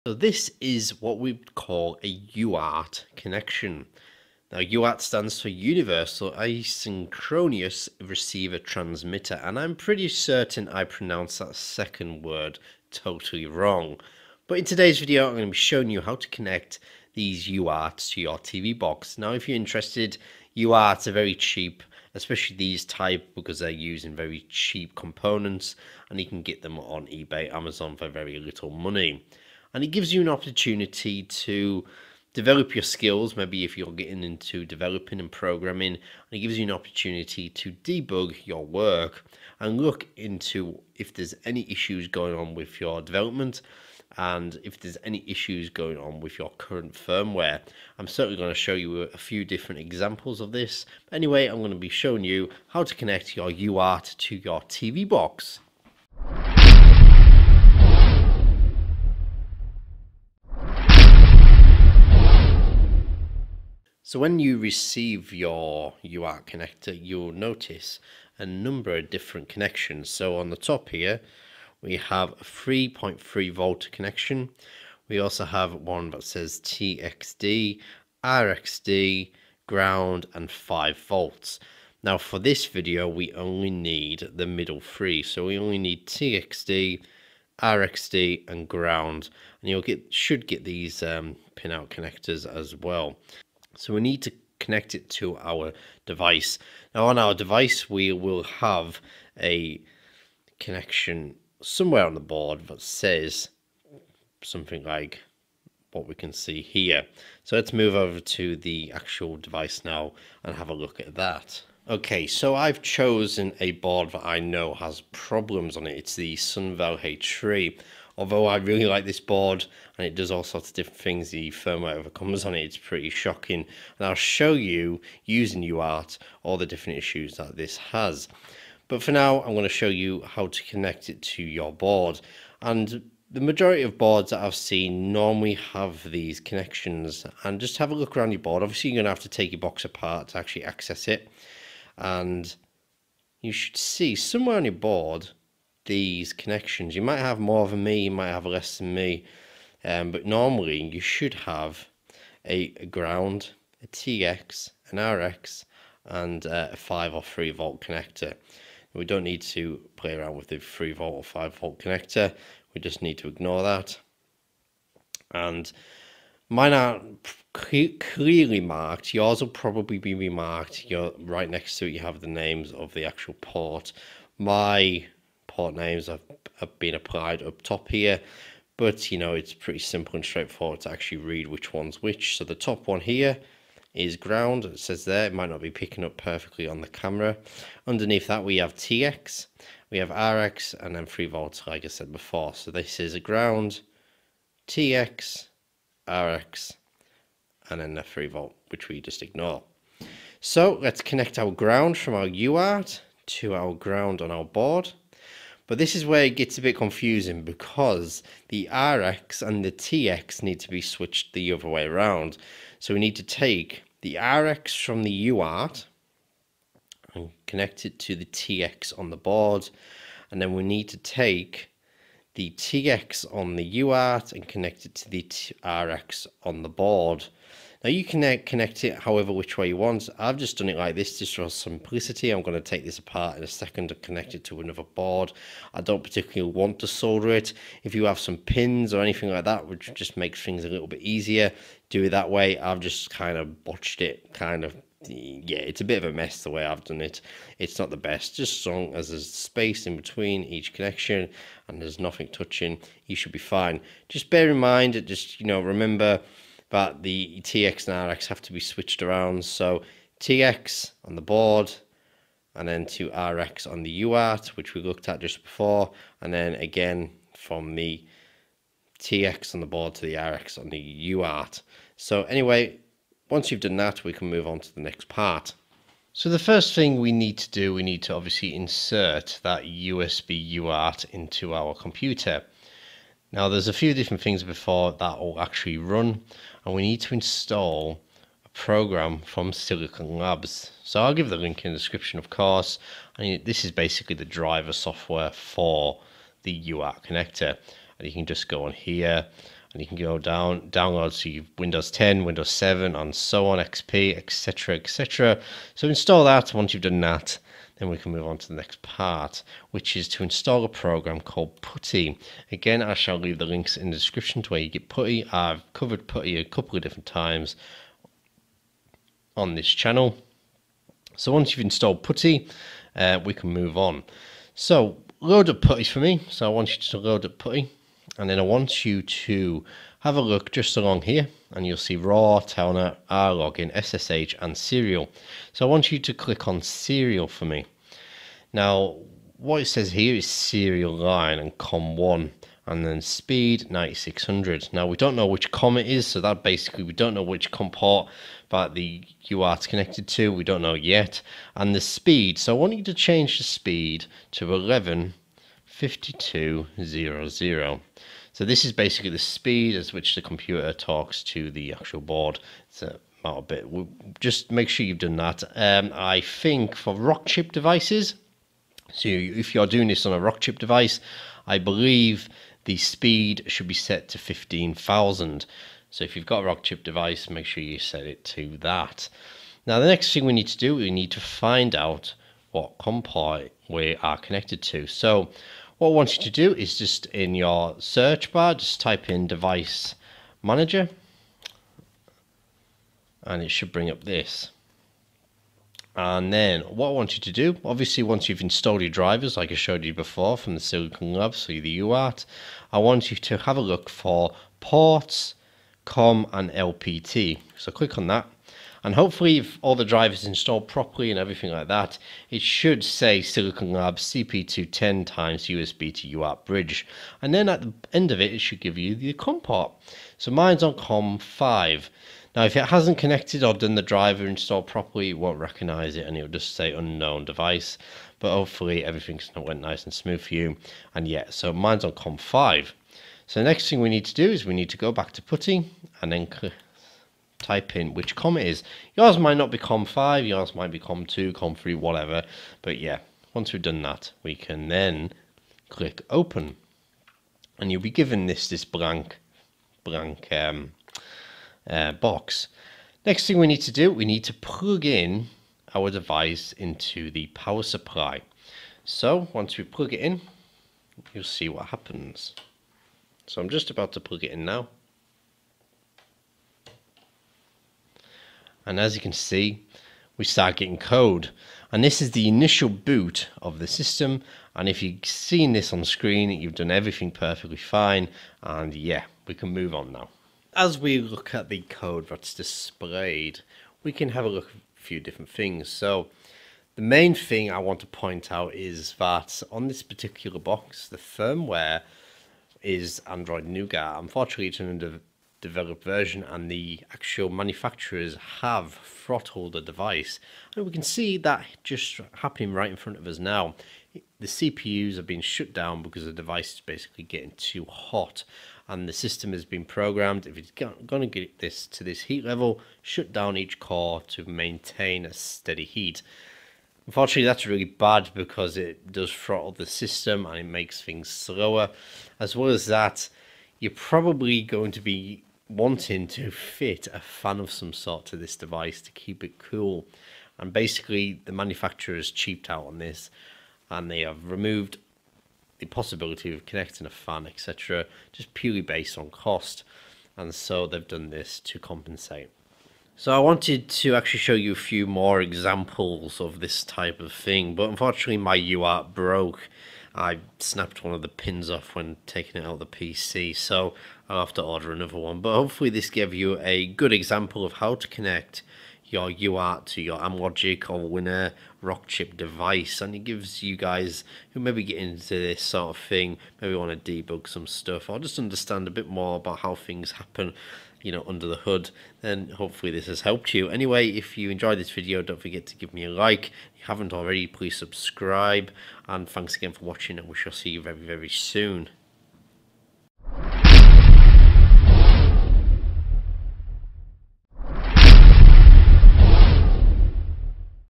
UART for Amlogic Allwinner and Rockchip Android TV Boxes: How To Connect Guide Tutorial
How to connect your USB UART Debug tool to your Android TV Box. This tutorial is aimed at Windows users wanting to connect their Amlogic, Allwinner or Rockchip device to UART to debug.

Budget Friendly TV box